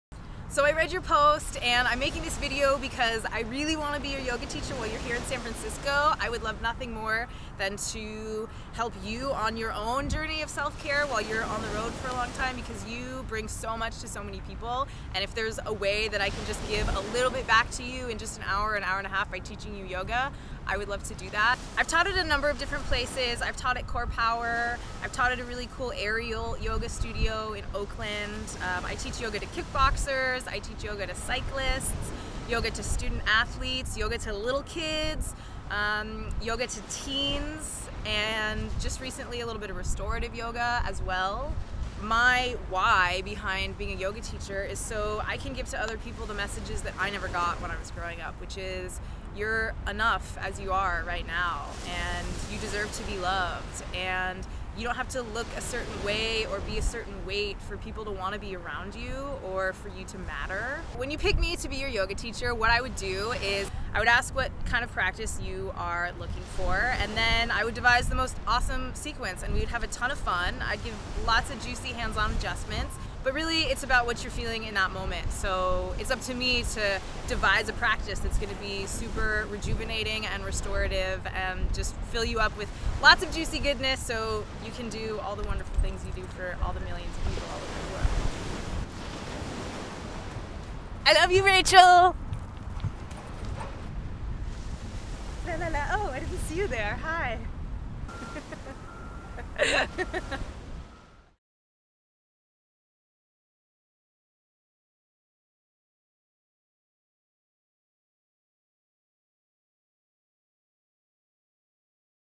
Helping Your Self-Care on the Happiness Tour
What you can expect when you take a Molly Pearson yoga class!